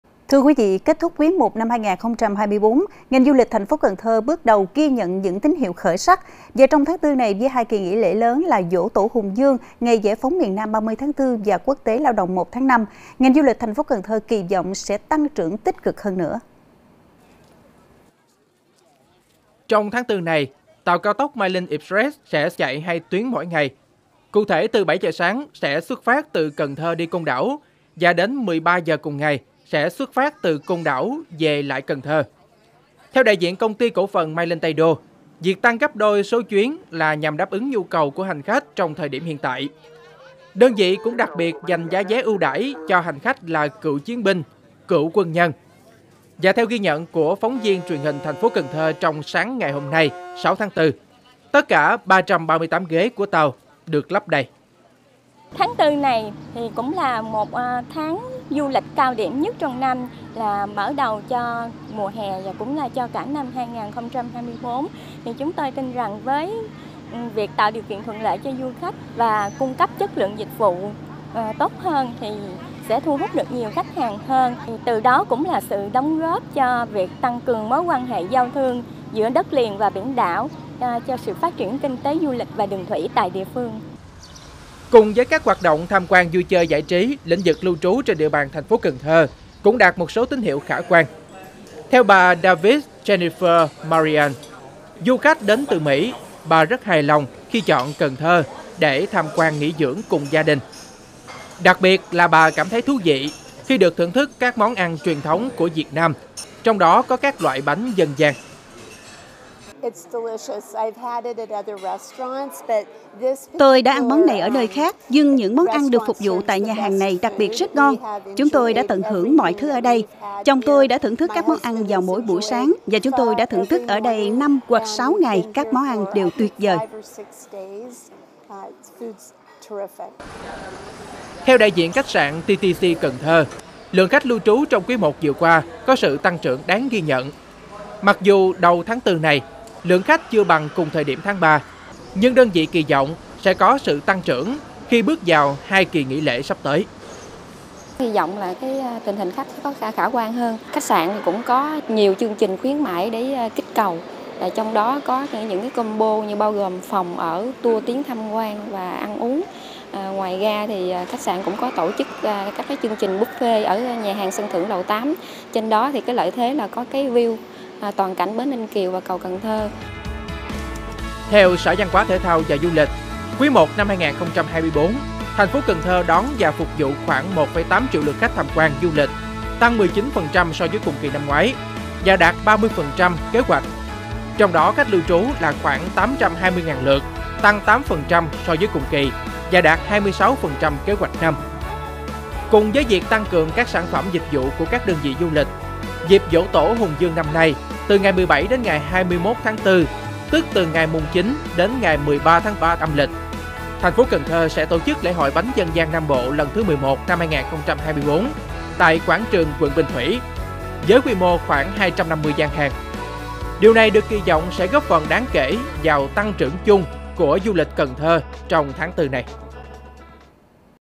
Du lịch Cần Thơ kỳ vọng tăng trưởng trong tháng 4| Cần Thơ TV
Kết thúc quý 1/2024, ngành du lịch TPCT bước đầu ghi nhận những tín hiệu khởi sắc. Và trong tháng 4 này, với 2 kỳ nghỉ lễ lớn là Giỗ tổ Hùng vương, Ngày giải phóng miền Nam 30/4 và Quốc tế Lao động 1/5, ngành du lịch Cần Thơ kỳ vọng sẽ tăng trưởng tích cực hơn nữa.